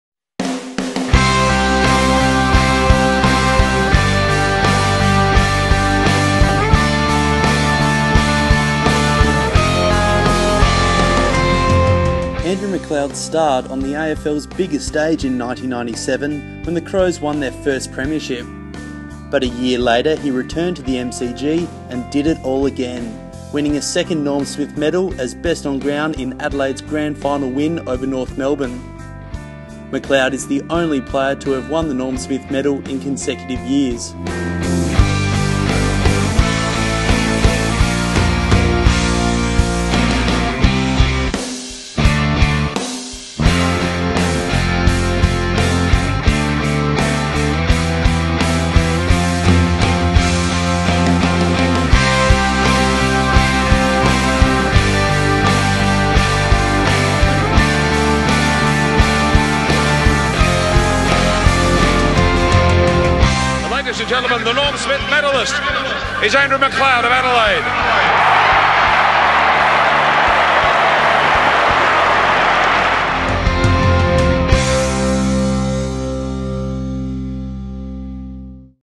Greatest Moments: #6
Andrew McLeod's second Norm Smith Medal is #6 in the Crows Greatest Moments.